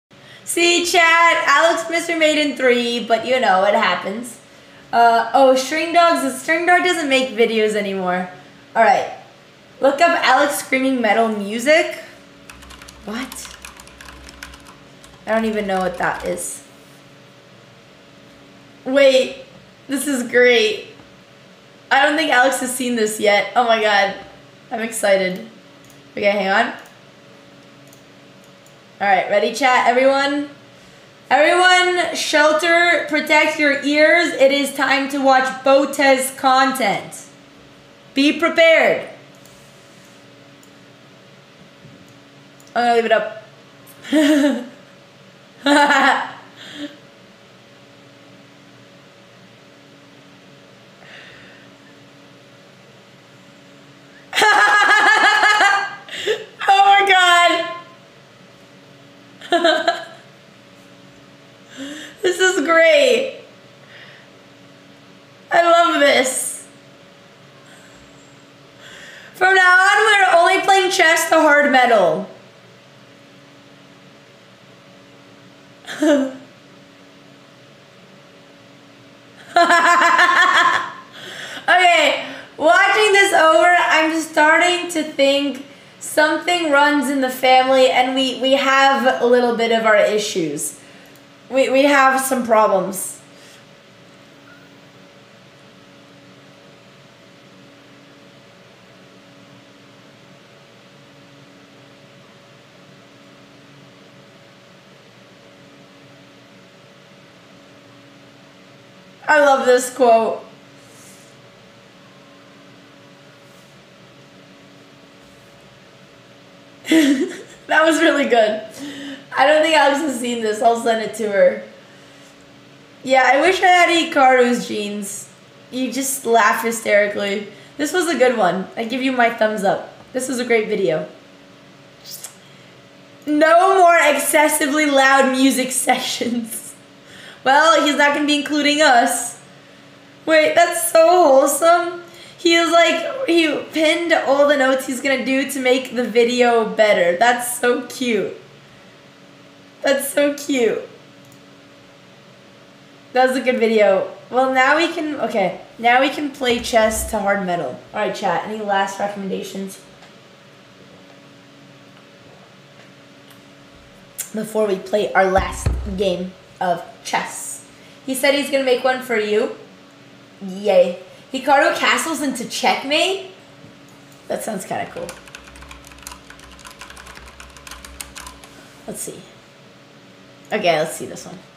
Andrea Botez looks up "Alex screaming metal music" at chat's request, and reacts
[UNEDITED CLIP a.k.a. low-effort content]

for the context on the quote i added in in the beginning of the description of this video
i figured that if someone's eventually gonna upload a full clip of her reaction to that messy edit, why not have that uploader be its creator himself?

date of upload of video that warranted this context: 2023/04/16

the title is deliberately worded this way.

sidenote: Alex had, if i'm not wrong, seen the video before this stream, and had given it her approval in a comment via the @BotezLiveClips channel.